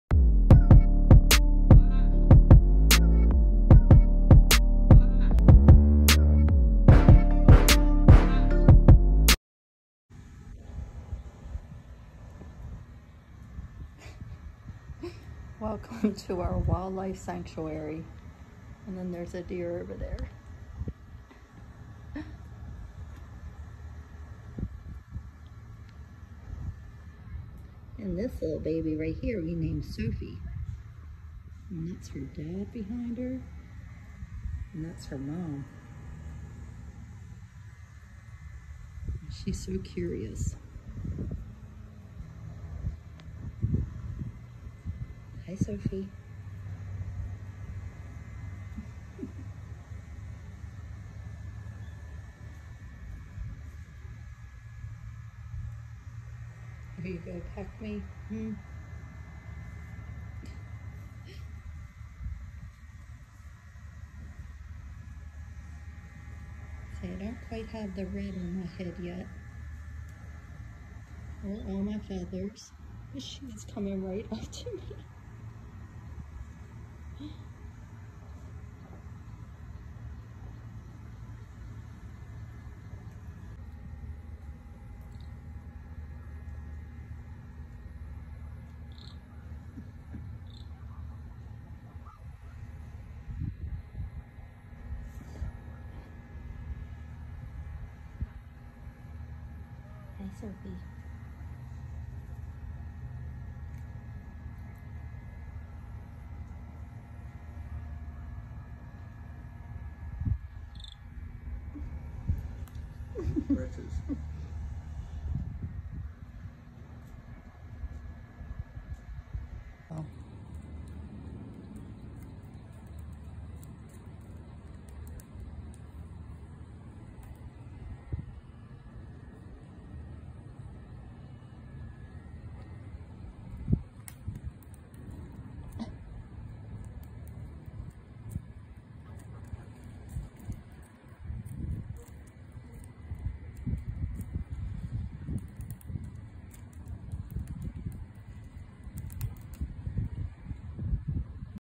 Florida Life! Wildlife Sanctuary In Our Backyard Cuteness Overload
We are so fortunate to be able to enjoy the wildlife from our back patio! Meet Sophia, she is a very young Sandhill Crane!  Her parents have been visiting our backyard since we moved here back in November!  The past month or so we weren’t seeing them around!  We thought that maybe they had a baby!  Well, this morning we found out we were right! They came back this morning and brought their little one! She is so cute and curious!